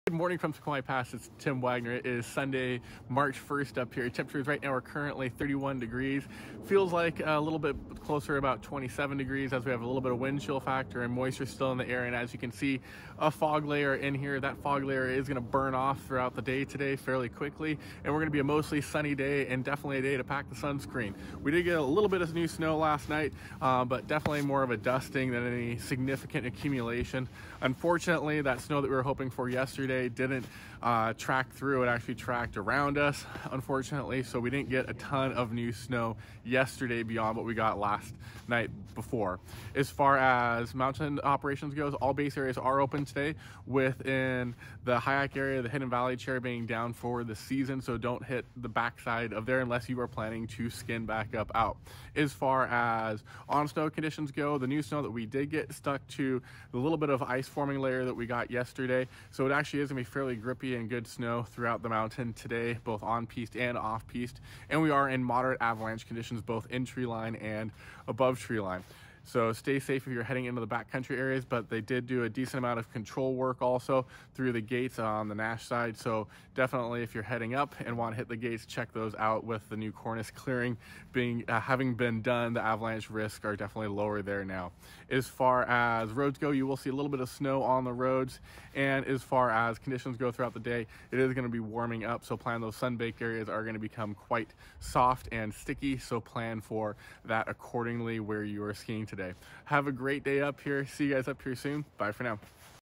A little new snow overnight.
Sunday, March 1st - We got about a inch of new snow overnight that has stuck well to the snow below. Due to some warmer weather than predicted there will be some ice patches but overall will be tracked powder. It will be sunny so pack the sun screen and loosening spring like now in areas that get sun baked later in the day.